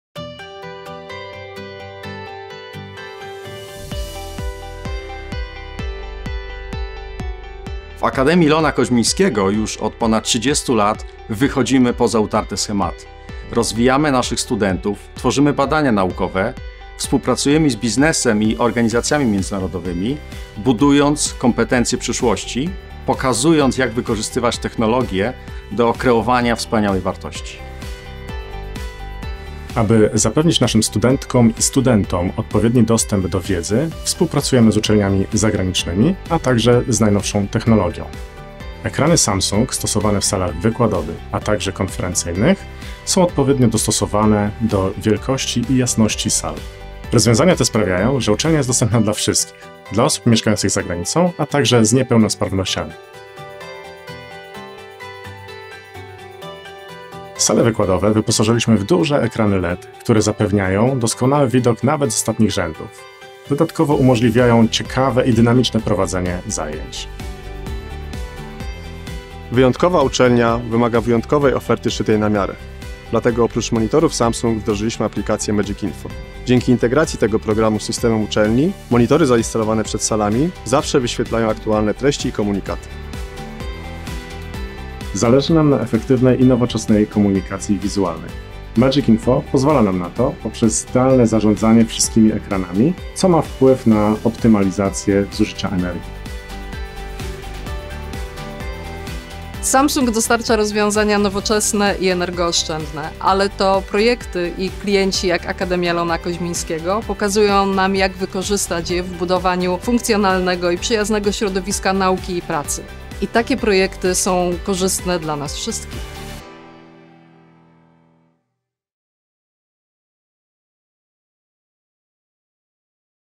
A new era of learning in Kozminski University | Samsung
Kozminski University’s the winner of Financial Times Master in CEE. Check how Samsung helps Kozminski University to design modern high-quality environment. Whether inside the classroom, around campus, or learning and teaching from home, Samsung’s education line-up gives teachers the tools they need to deliver an enhanced experience, while maintaining student engagement, collaboration and confidence.